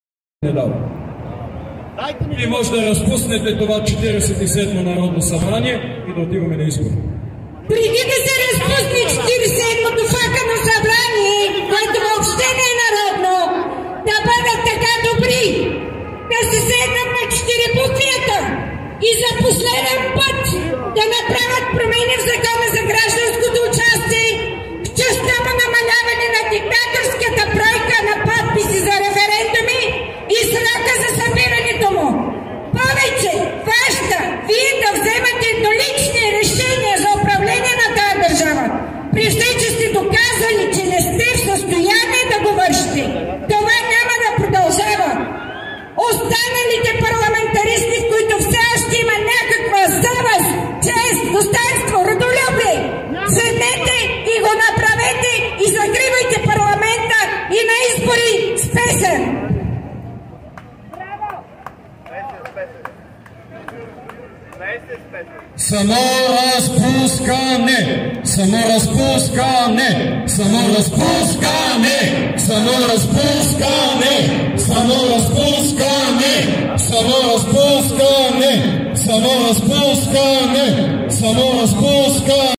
Агора България ... протест Промяна 2 - Не! пред Фонтана пред Президентството в гр. София
Събитие от Агора България и Агора БЪЛГАРИЯ
Фонтана пред Президентството на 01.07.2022 г.

Протестиращите заявиха - кратко, точно и ясно, че
кабинет "ПОДМЯНА" 2 - КАТЕГОРИЧНО, НЕ

Корупцията убива!
Безхаберието убива!
Двойният стандарт убива!